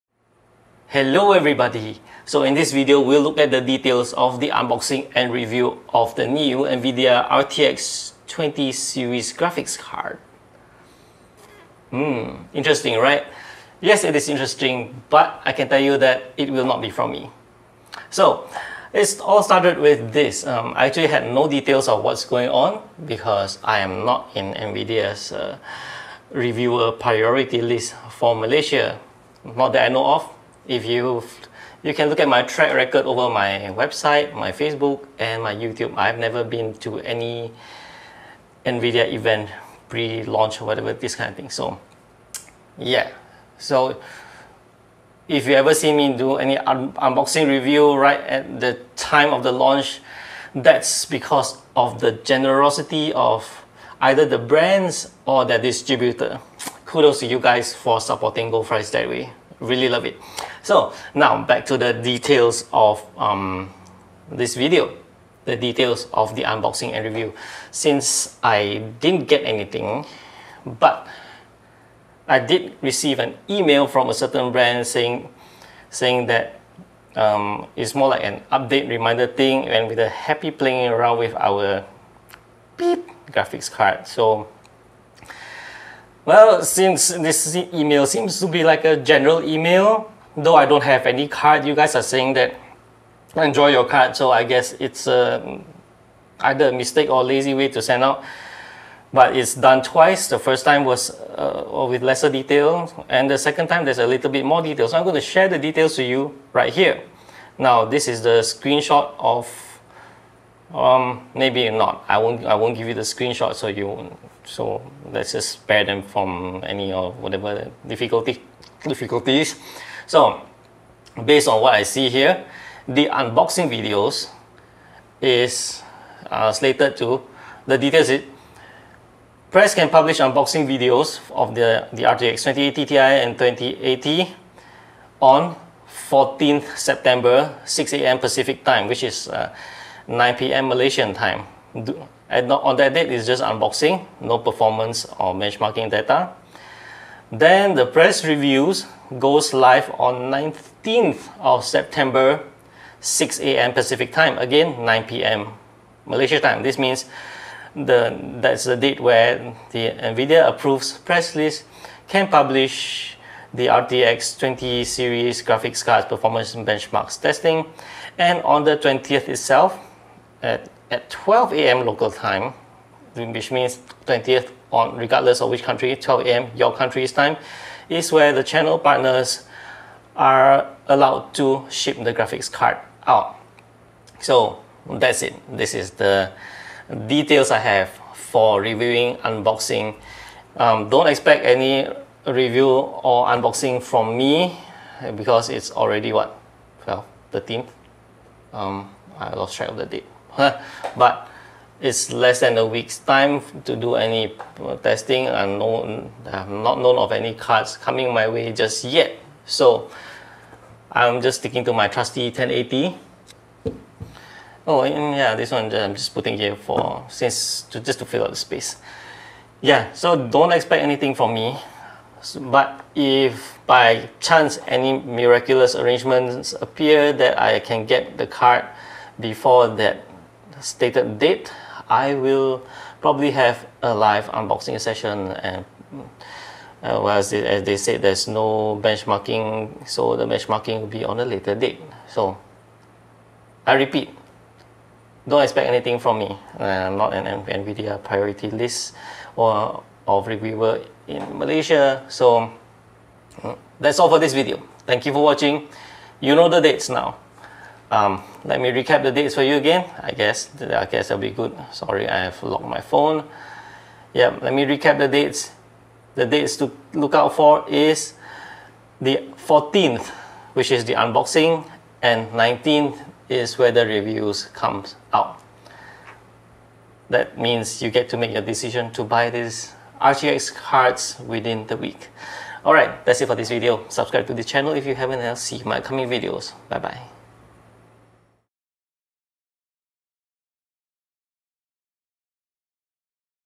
Nvidia RTX 2080 Ti and RTX 2080 - Dates and Details to know for Unboxing, Reviews and Availability
Want performance details, benchmarks and unboxings of the RTX 2080 Ti and RTX 2080 by Nvidia? You need to know these dates!

- Unboxing goes live on 14th September 2018 - 9PM GMT
- Reviews goes live on 19th September 2018 - 9PM GMT

And no, don't expect anything from me as there's no card heading my way. No arrangements at the moment.